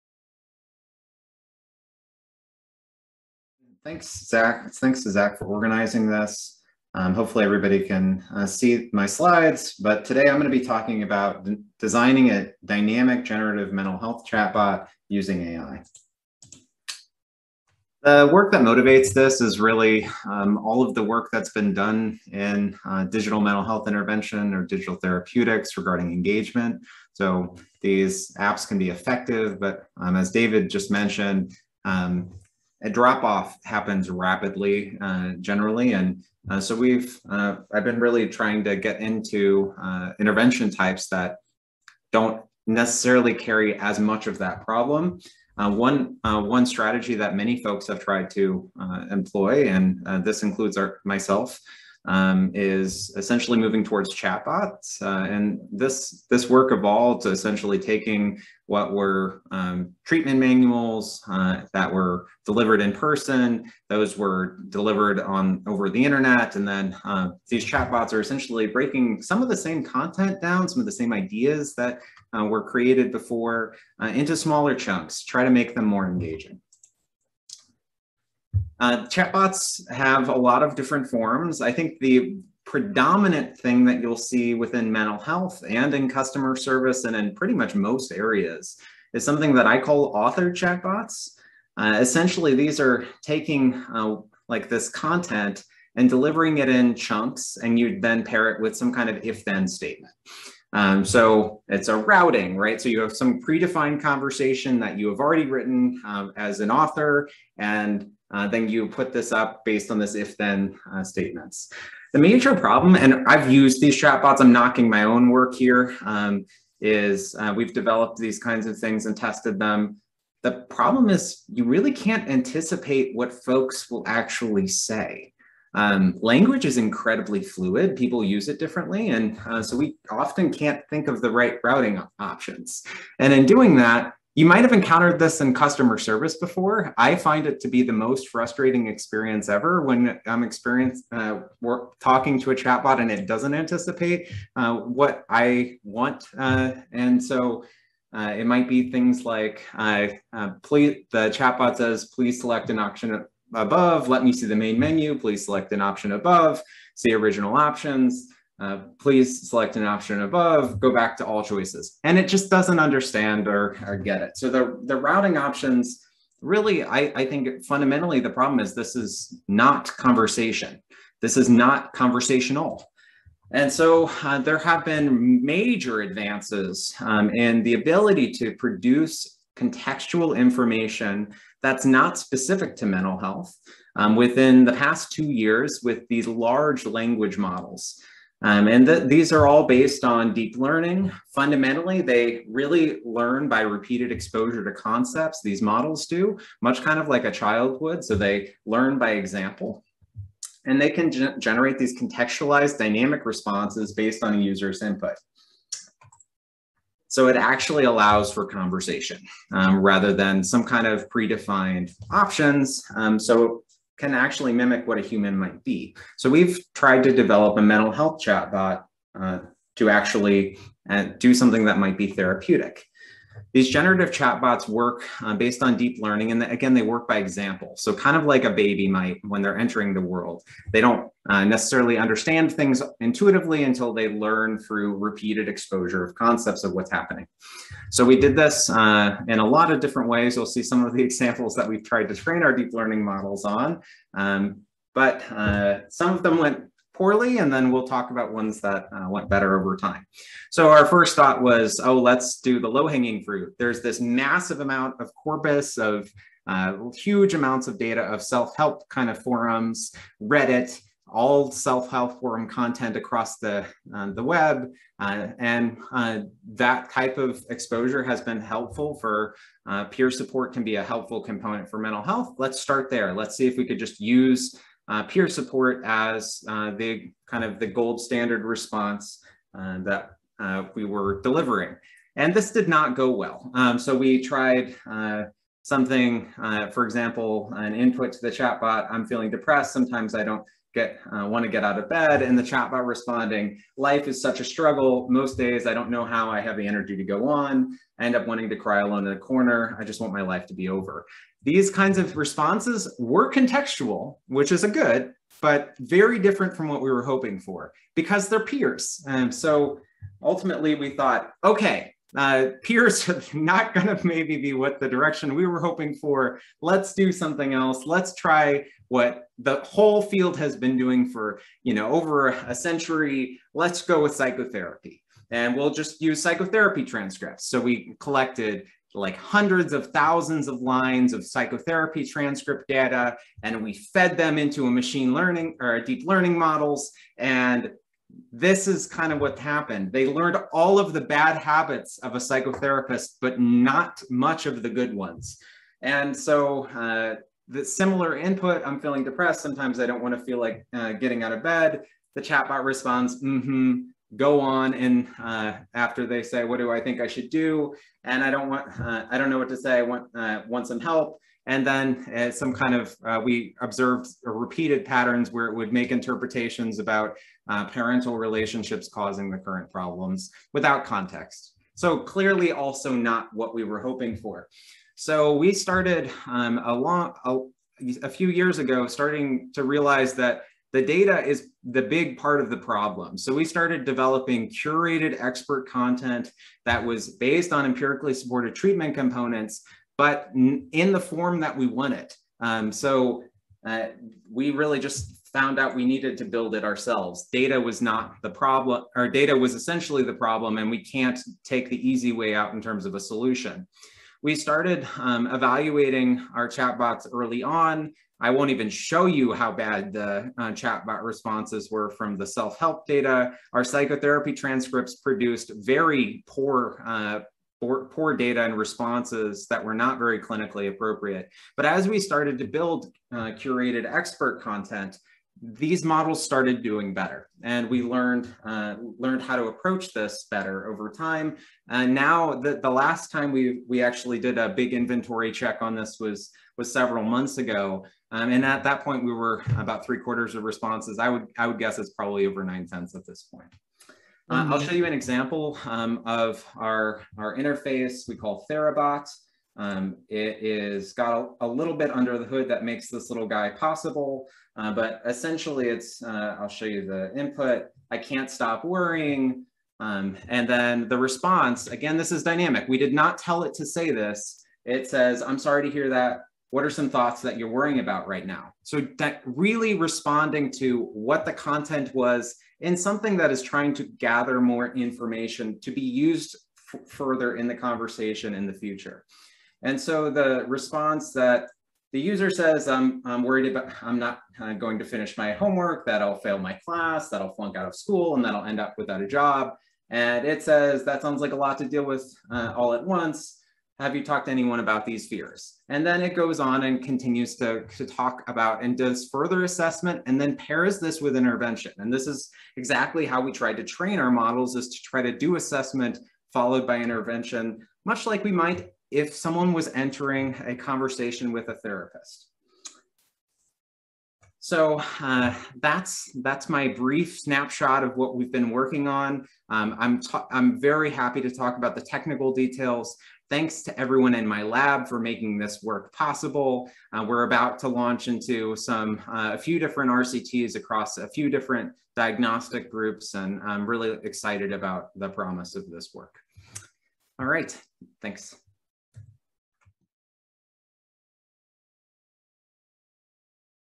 Nicholas Jacobson: "Designing a Dynamic Generative Mental Health Chatbot Using AI"
to learn more about the McLean Institute for Technology in Psychiatry.

Session Details: "Improving the Effectiveness and Scalability of Digital Mental Health Through Precision and Personalization"

Digital therapeutics are needed to expand access to evidence-based interventions for the prevention and treatment of mental health problems. However, much work remains to ensure that these treatments are engaging and effective. Precision approaches can help patients receive the right interventions at the right time, and personalization approaches can tailor therapeutic content to the user. In this symposium, seven leading digital mental health researchers will present their work in the areas of digital therapeutics, digital phenotyping, and dissemination and implementation of digital therapies. They will discuss how they are working to move beyond the “one size fits all” approach to treatment, by using AI to tailor messages and deep learning to develop generative chatbots; they will discuss how novel data from wearables and mobile devices can be used to track and adapt digital interventions including behavioral activation, cognitive therapy, and prolonged exposure; and they will review unique challenges they have encountered developing web- and smartphone-based therapies across a variety of disorders, settings and populations.

Olusola Ajilore, MD, PhD – Professor, University of Illinois-Chicago
Nicholas B. Allen, PhD –Ann Swindells Professor of Clinical Psychology, University of Oregon
Nicholas Jacobson, PhD – Assistant Professor, Geisel School of Medicine Dartmouth College
Debra Kaysen, PhD – Professor, Stanford University Medical Center
Carmen McLean, PhD – Clinical Associate Professor, National Center for PTSD, Palo Alto VA Health Care System
David C. Mohr, PhD – Director, Center for Behavioral Intervention Technologies, Northwestern University
Shannon Wiltsey Stirman, PhD – Associate Professor, Stanford University School of Medicine
Moderator: Zachary Cohen, PhD – Researcher, University of California, Los Angeles